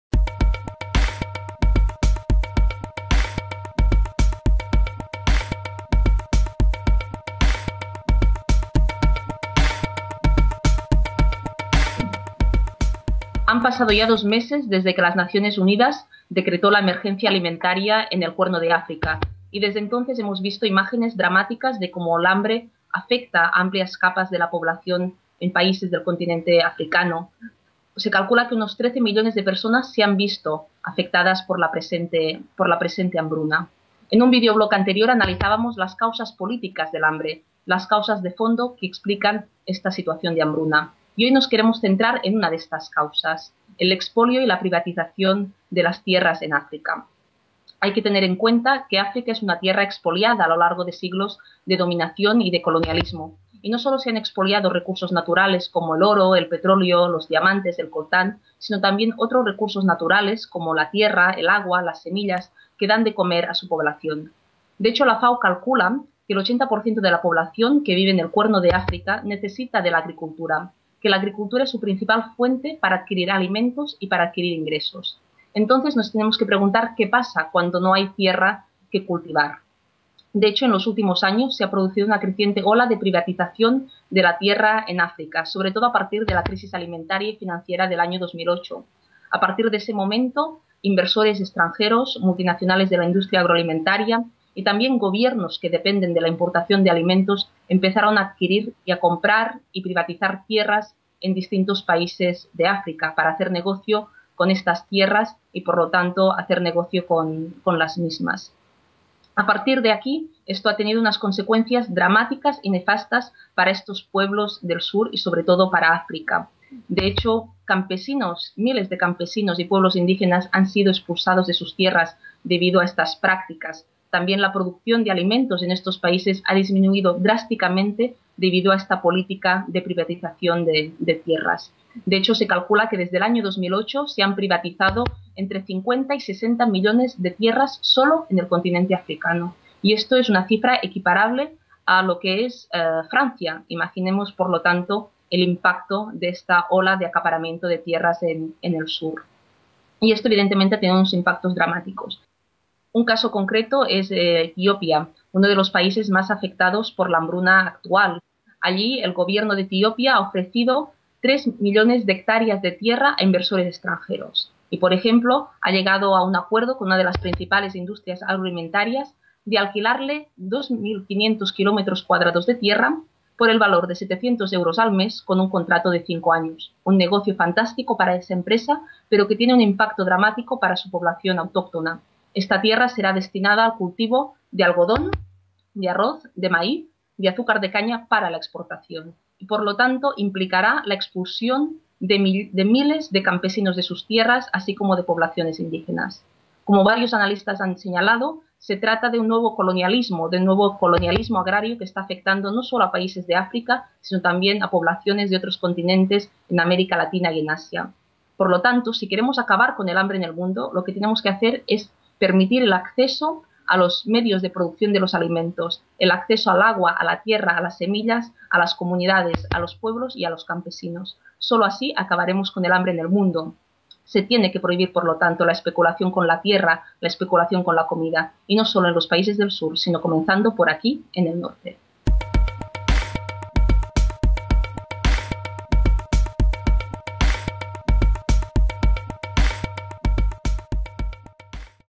Causas del hambre en África; expolio y privatización de la tierra
Como continuación del Videoblog "Especulación Alimentaria=HAMBRE" Esther Vivas habla de las causas políticas del hambre; la expoliación y privatización de las tierras en África.